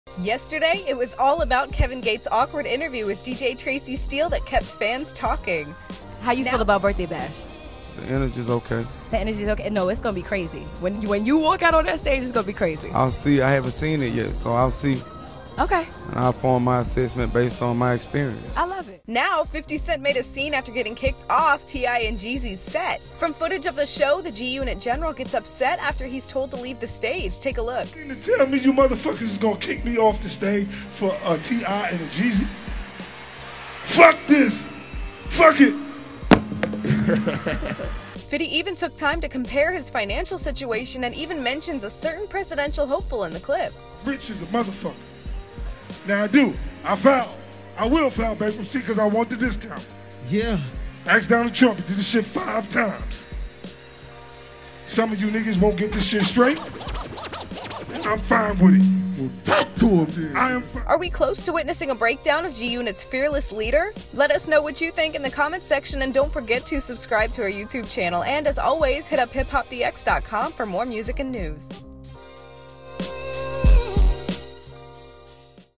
50 Cent’s Rants After Being Asked To Leave Stage
Last weekend’s 107.9's Birthday Bash is still making news.